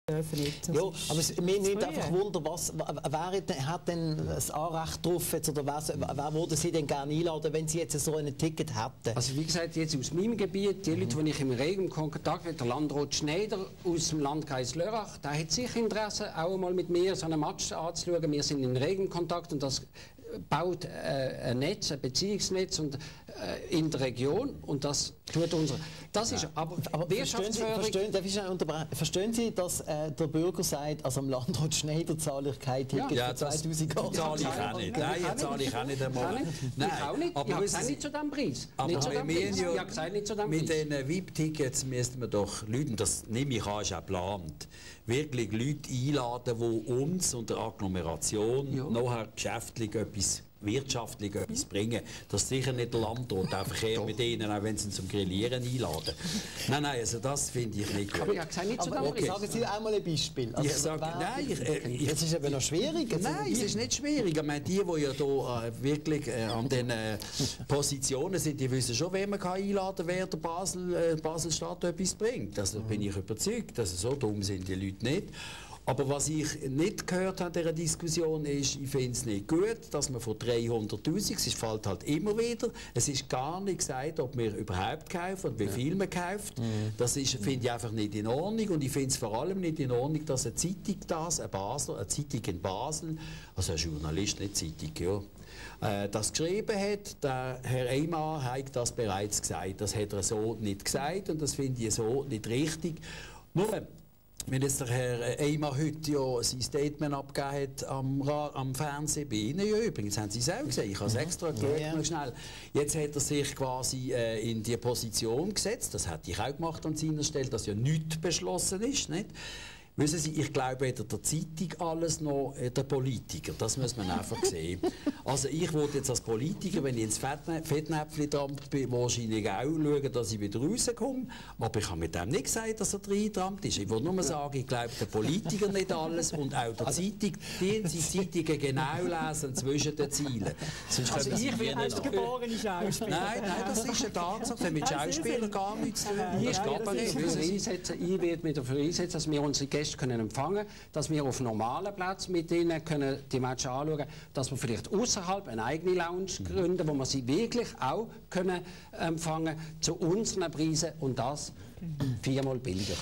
Aussage Telebasel Herr Hersberger über Regierungsrat Eymann
Aussage Herr Hersberger über Herr Eymann